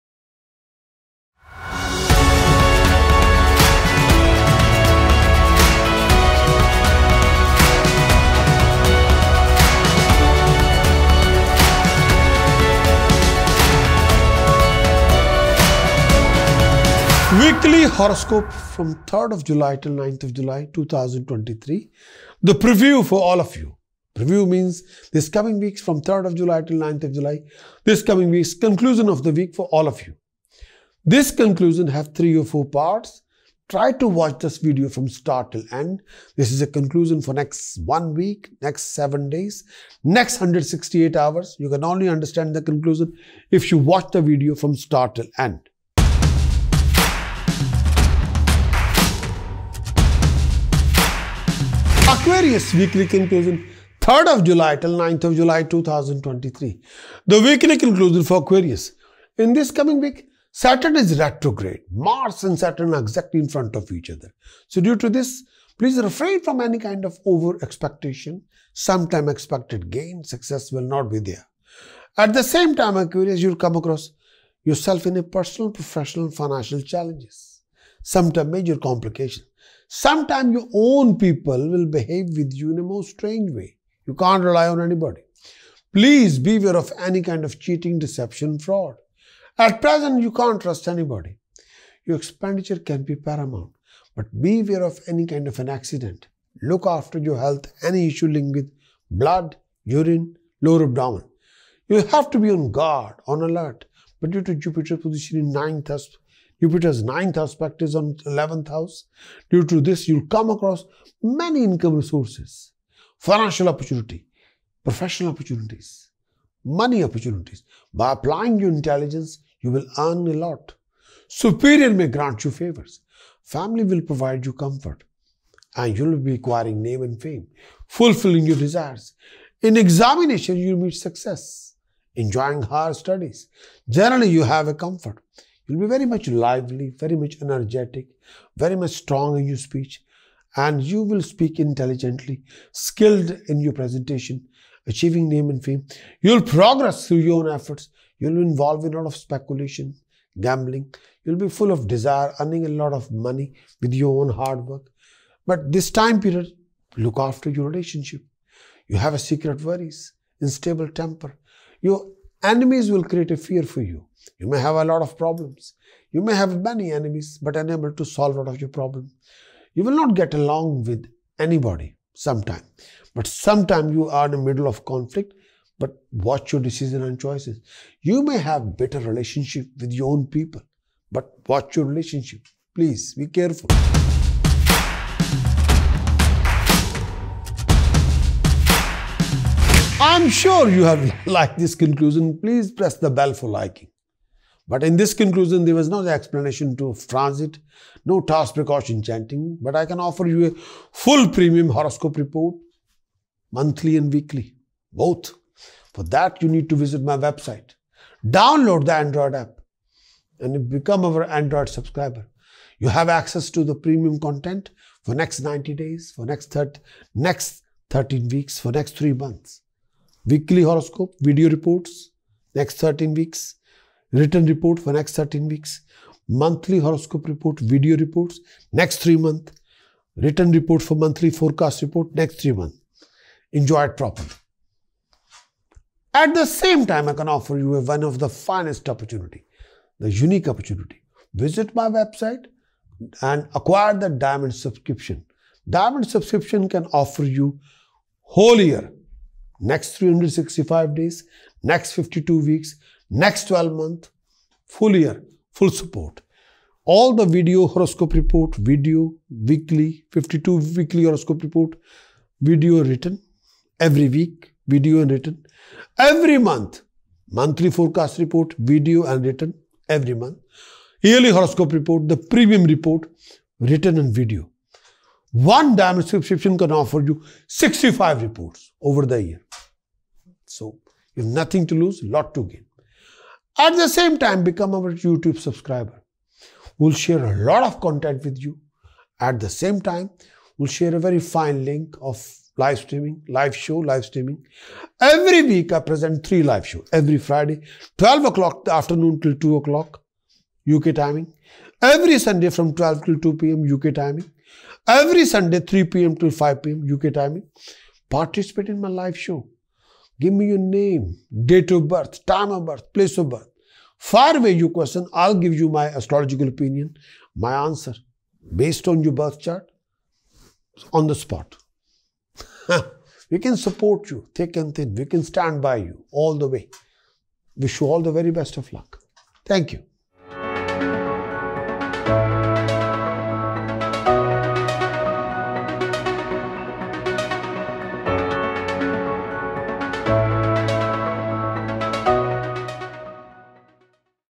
Aquarius Weekly Horoscopes Video For 3rd July 2023 | Preview
Here is your 3rd July Weekly Horoscope. In this video we will be covering your health, education, romance, relationship, career, job, profession and conclusion within this Weekly Forecast. Please note that this horoscope video is based on your Moon Sign and does not include Tasks, Precautions and Chanting.